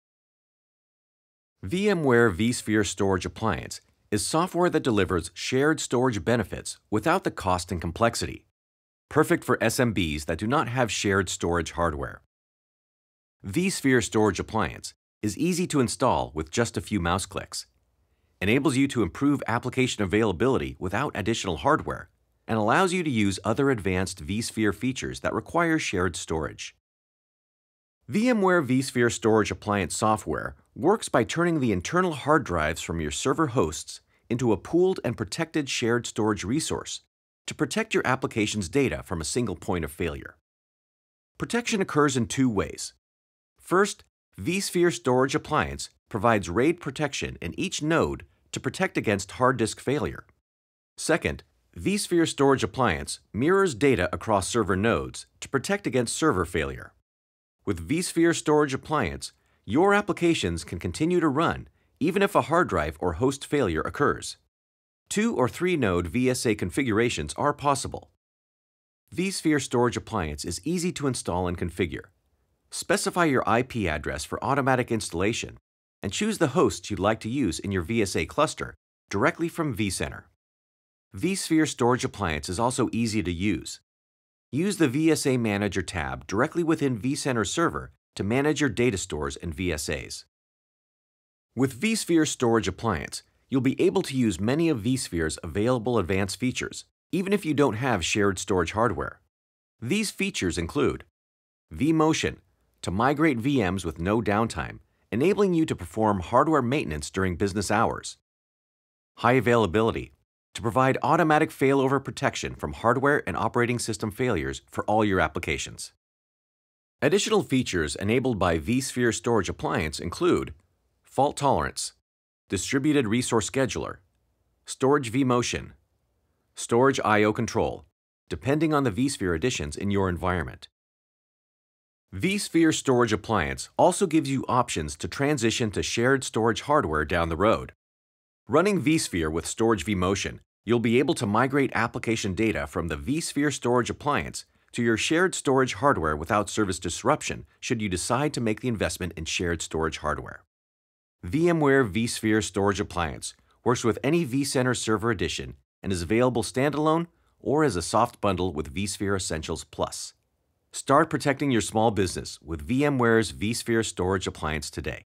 VMware vSphere Storage Appliance Overview
-- Software that delivers shared storage benefits for SMBs without shared storage hardware. VMware vSphere Storage Appliance extends the benefits of virtualization to all SMBs by transforming internal storage from several server hosts into a protected shared storage resource.

Visit "VMware NOW" -- our new on-demand digital experience offering VMware keynotes, product videos and social media collaboration